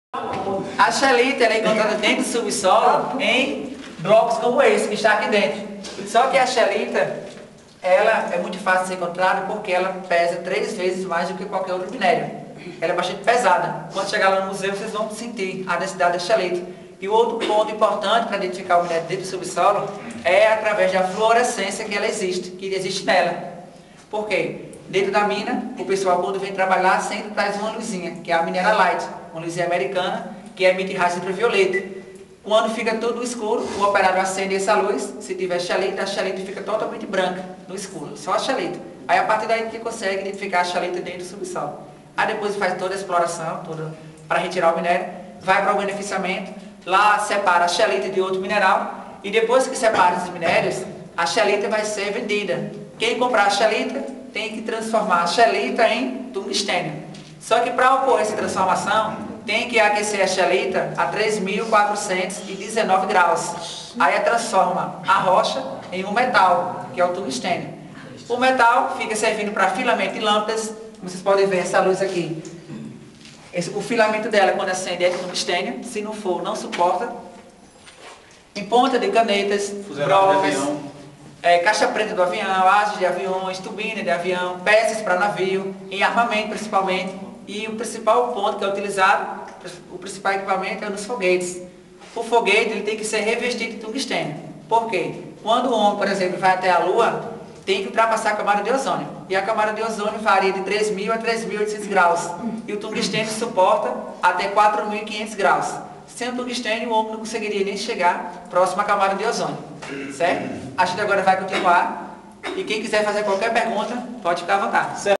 IFRN - AULA DE CAMPO EM CURRAIS NOVOS MINERADORA BREJUÍ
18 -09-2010 TURMA DE GUIA DE TURISMO E SEGURANÇA DE TRABALHO - PÓLO PARNAMIRIM NA MINA BREJUÍ